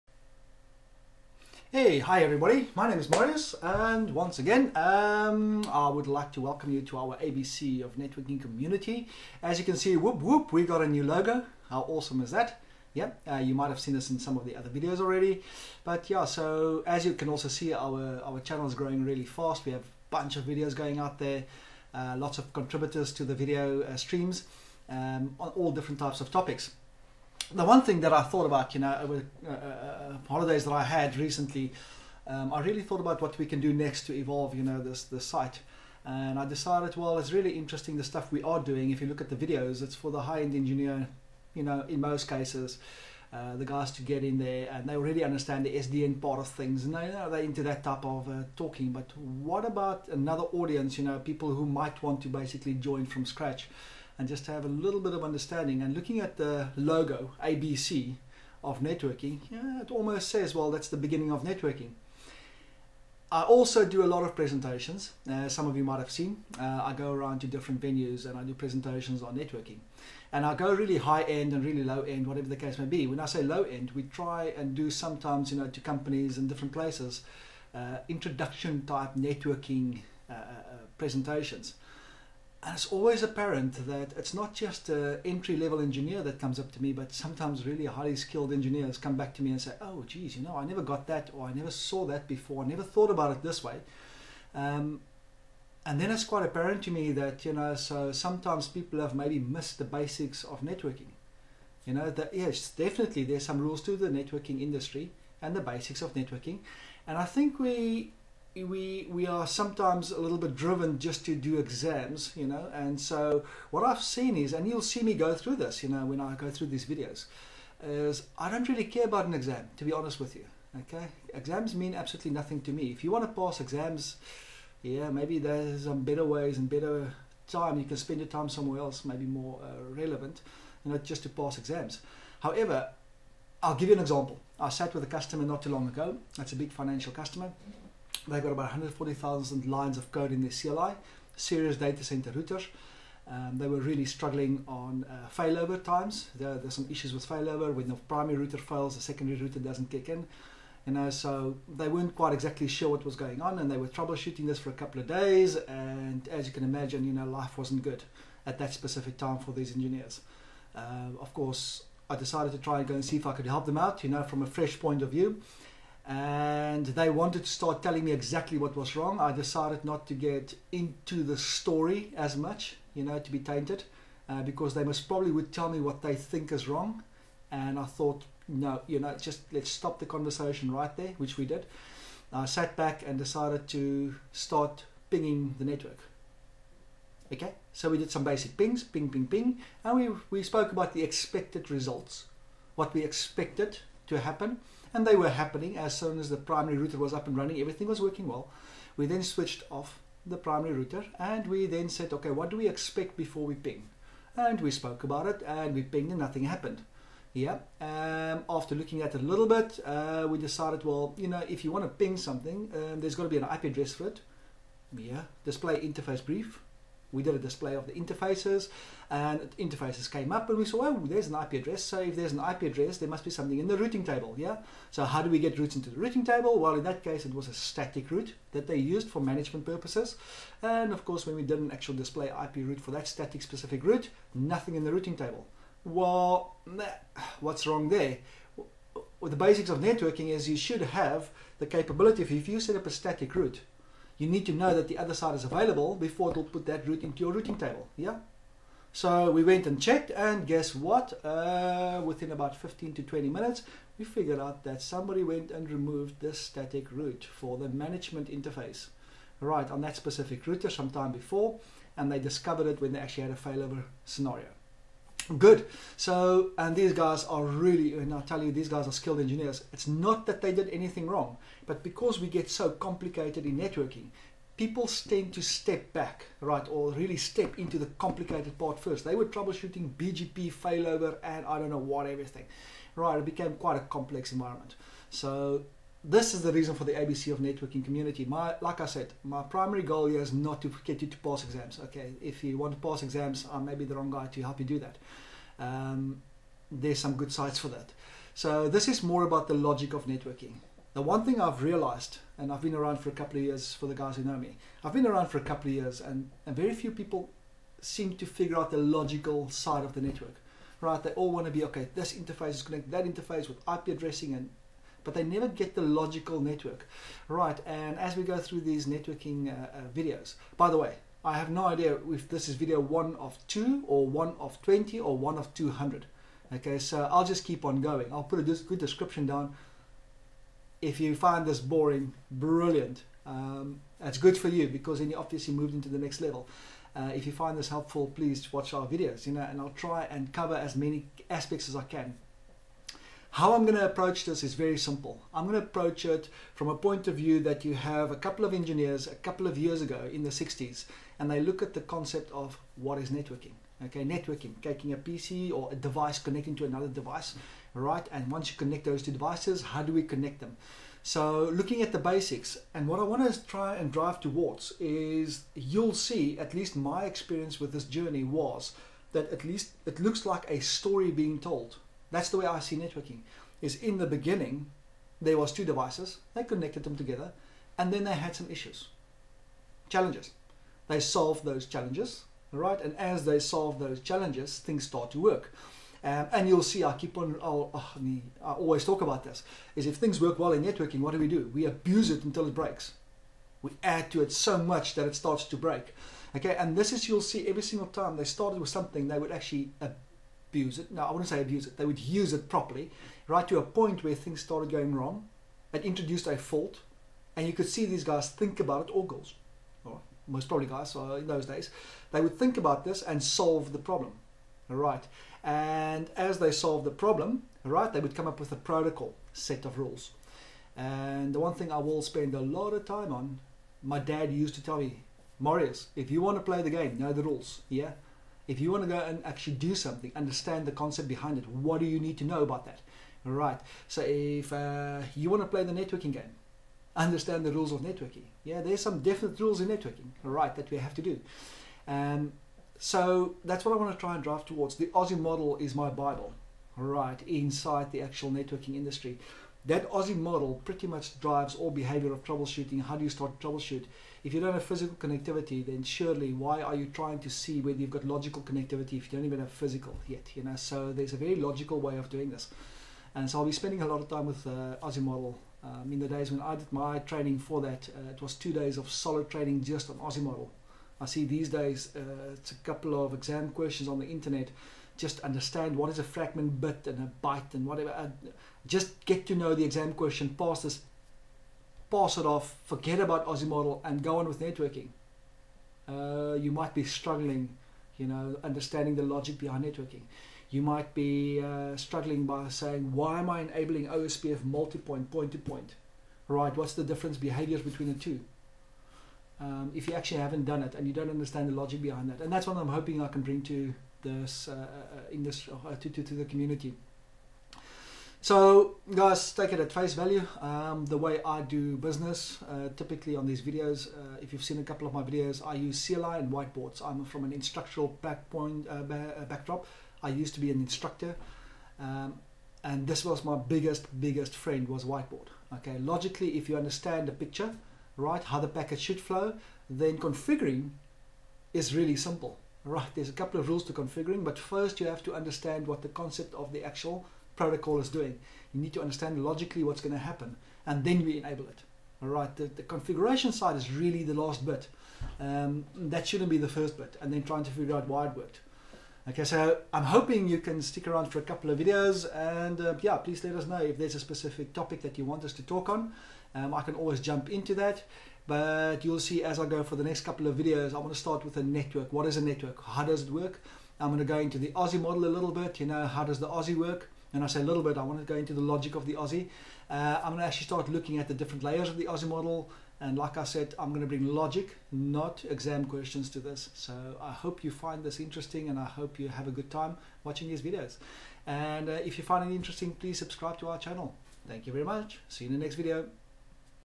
ABC Networking introduction Video 1
This Video is an introduction to networking basics. These Videos are intended for people who want to understand the logic of networking concepts. These videos can also be used as refresher videos for current Engineers but also a great introduction to everyone else.